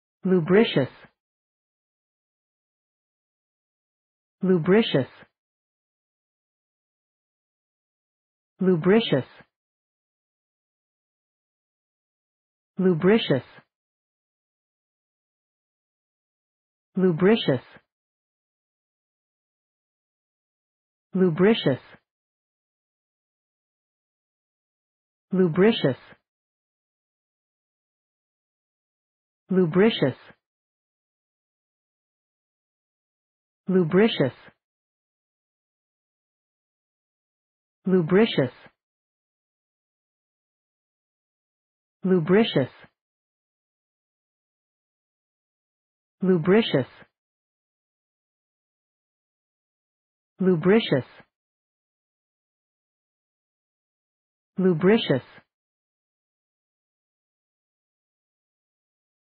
Pronunciation of Lubricious | Definition of Lubricious
Lubricious pronunciation | How to pronounce Lubricious in English?
/luː`brɪʃəs/
Meaning of Lubricious | What is Lubricious?
(1) (adjective) characterized by lust; (2) (adjective) having a smooth or slippery quality;
What is Lubricious | Lubricious meaning and definition?